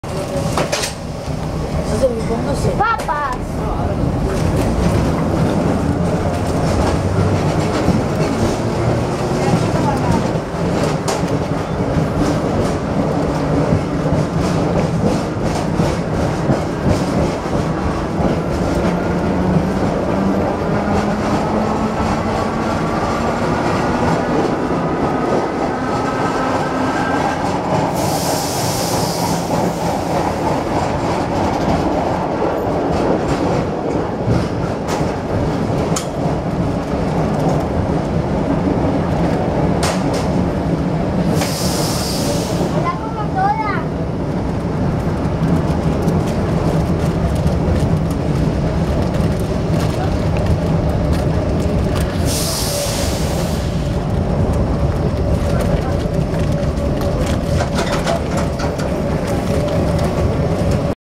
Subte Linea A - De Plaza de Mayo a Perú (HD)
Una formación La Brugeoise sale de la estación Plaza de Mayo para detenerse en Perú rumbo a Carabobo.

Los coches La Brugeoise son los trenes en servicio comercial más antiguos del mundo y parte del patrimonio histórico y cultural argentino.